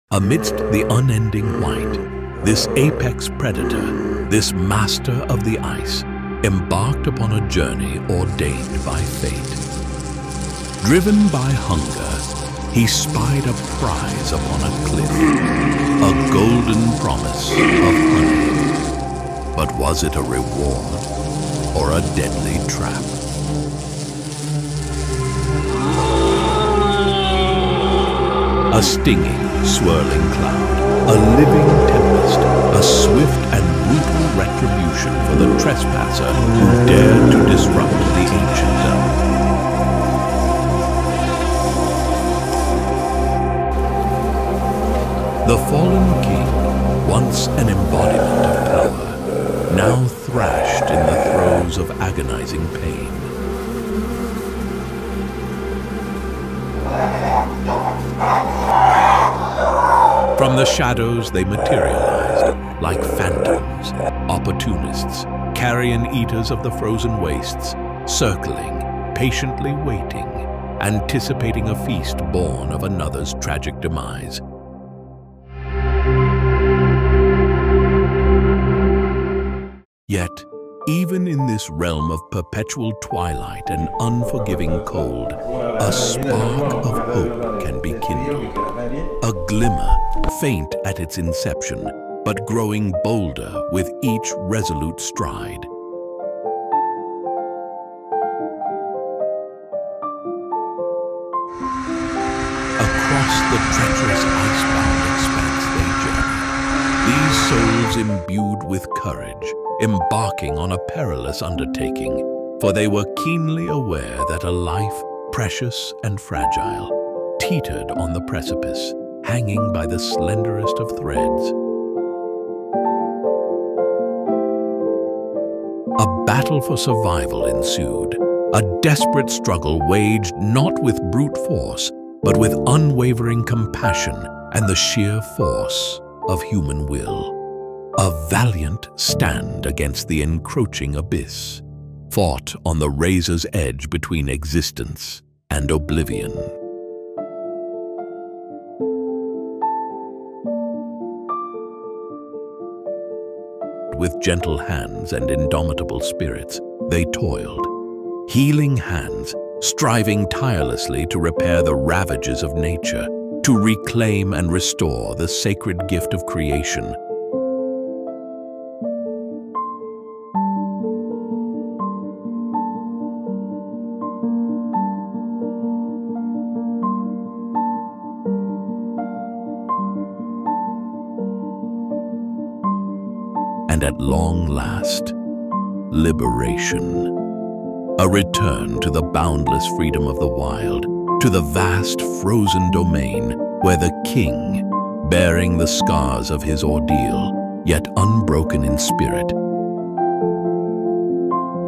The Disturbing Reason Polar Bears Are Losing Their Fur
Witness the incredible journey of a polar bear's rescue from a devastating parasite infestation, a testament to the power of bear rehabilitation and habitat conservation efforts. In the harsh Arctic environment, these majestic creatures face numerous threats, including the degradation of their natural habitat. This heartwarming rescue story highlights the importance of protecting and preserving their habitat, ensuring the long-term survival of polar bears and the ecosystem as a whole. Get ready to be moved by this remarkable tale of resilience and conservation, a beacon of hope for the future of these incredible animals.